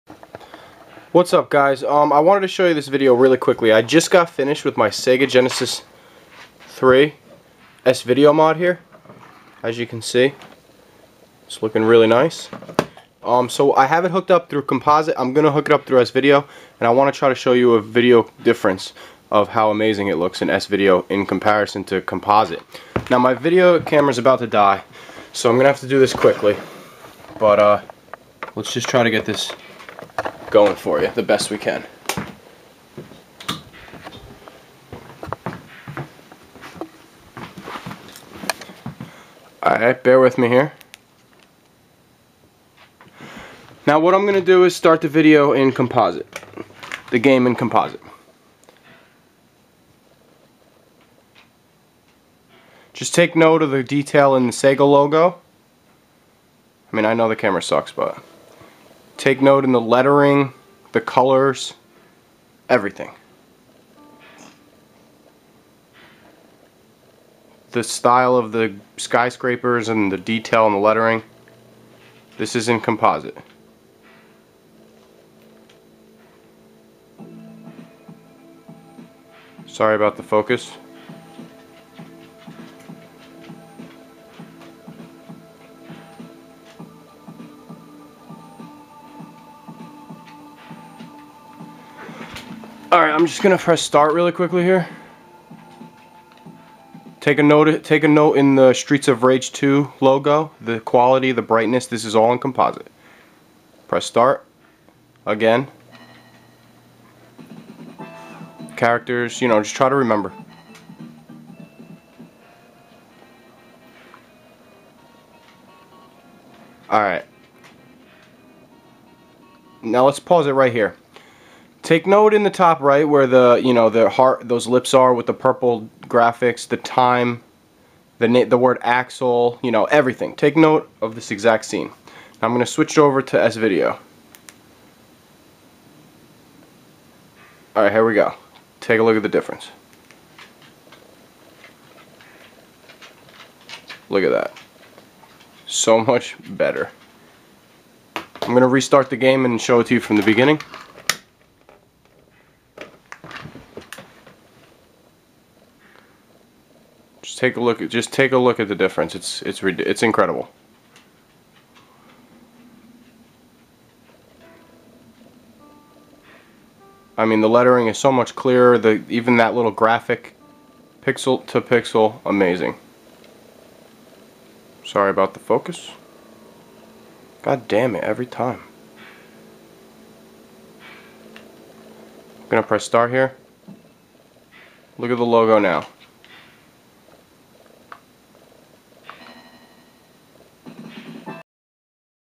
Genesis Model 3 S-Video Mod
modded my genesis 3 for svideo... my camera dies but you get the idea...